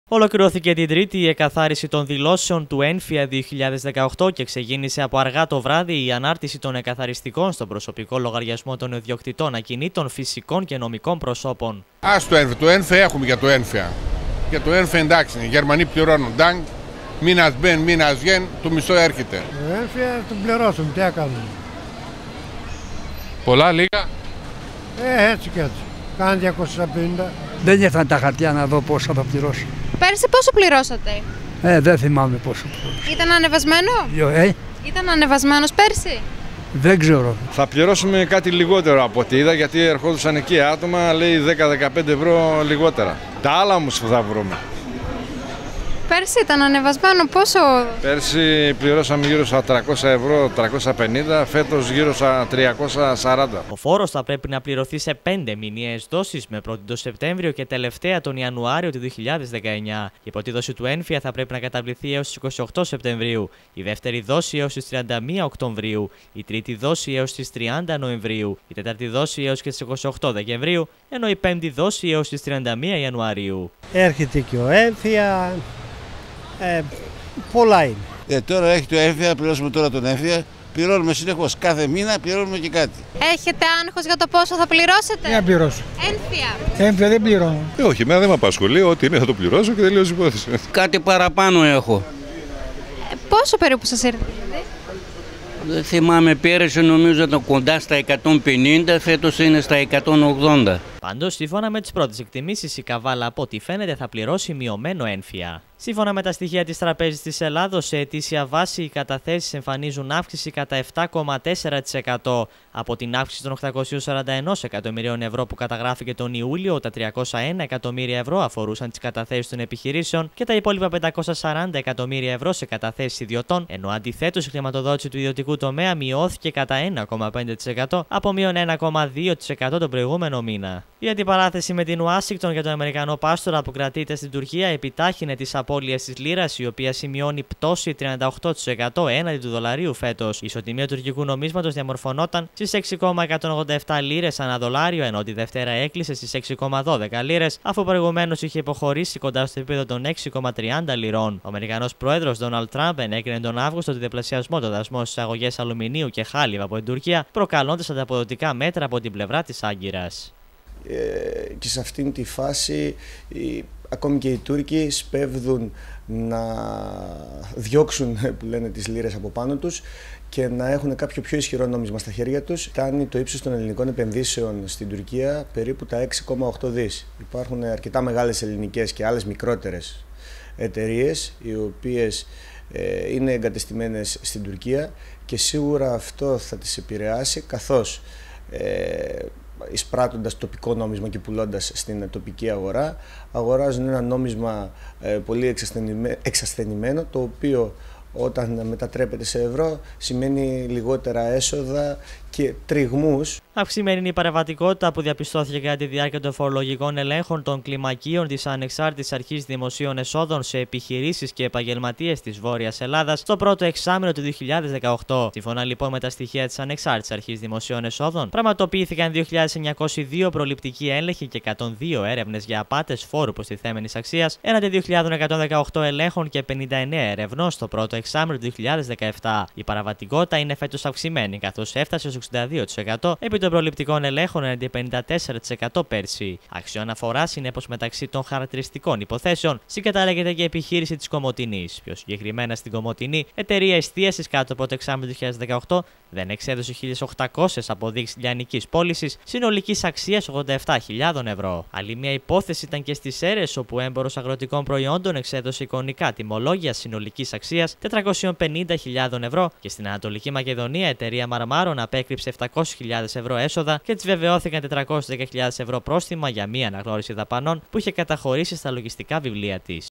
Αναρτήθηκε ο ΕΝΦΙΑ για το 2018 | Μέχρι τέλη Σεπτεμβρίου η πρώτη δόση
Ολοκληρώθηκε σήμερα η εκκαθάριση των δηλώσεων του ΕΝΦΙΑ 2018 και ξεκίνησε από χθες αργα βράδυ η ανάρτηση των εκκαθαριστικών στον προσωπικό λογαριασμό στο TAXISnet των ιδιοκτητών ακινήτων, φυσικών και νομικών προσώπων. Ήδη οι φορολογούμενοι σπεύδουν να μπουν στην πλατφόρμα της ΑΑΔΕ για να δουν τι θα πληρώσουν με τις νέες αντικειμενικές αξίες ενώ ξεκινούν και οι συμψηφισμοί